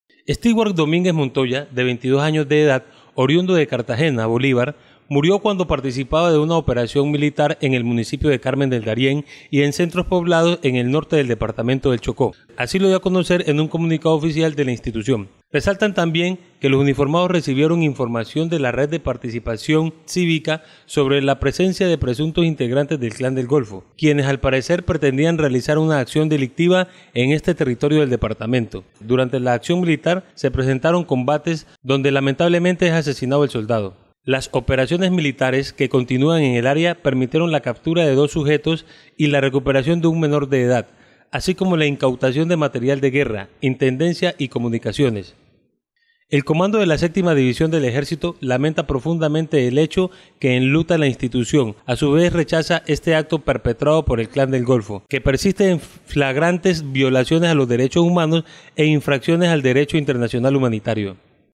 Soldado muere en combate con clan del golfo en Carmen del Darién
Estiwar Domínguez Montoya, de 22 años de edad, oriundo de Cartagena, Bolívar, murió cuando participaba de una operación militar en el municipio de Carmen del Darién y en centros poblados en el norte del departamento del Chocó, así lo dio a conocer en un comunicado oficial de la institución. Resaltan también que los uniformados recibieron información de la Red de Participación Cívica sobre la presencia de presuntos integrantes del Clan del Golfo, quienes al parecer pretendían realizar una acción delictiva en este territorio del departamento, durante la acción militar se presentan combates, donde lamentable es asesinado el soldado.